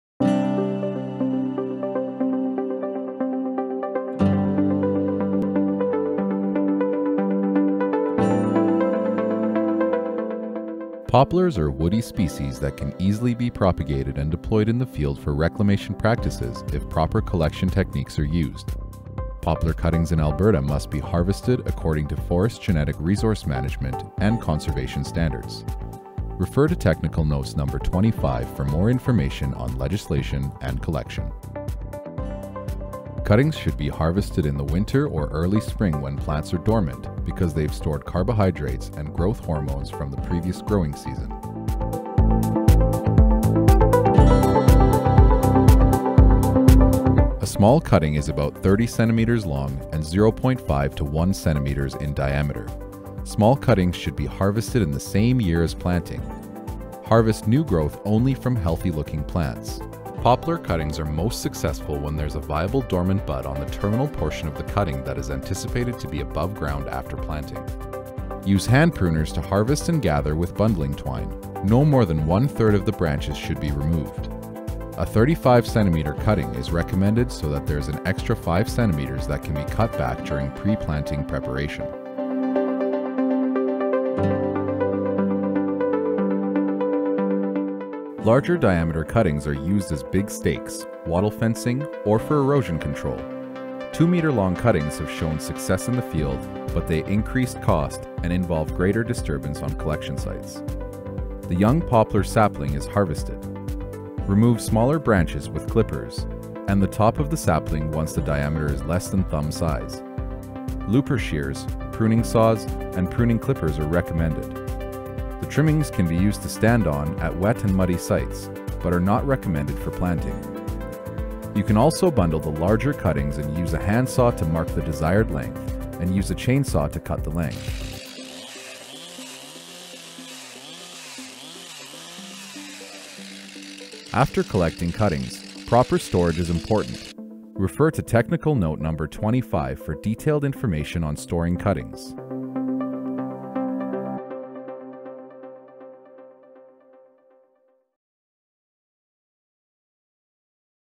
Field Collection of Balsam Poplar Cuttings
Balsam poplars (Populus balsamifera) is a woody species that can easily be propagated and deployed in the field for revegetation purposes if appropriate collection techniques are utilized. This technical video provides methods for field harvesting of both small (30 cm length) and large (up to 2 m length) poplar cuttings.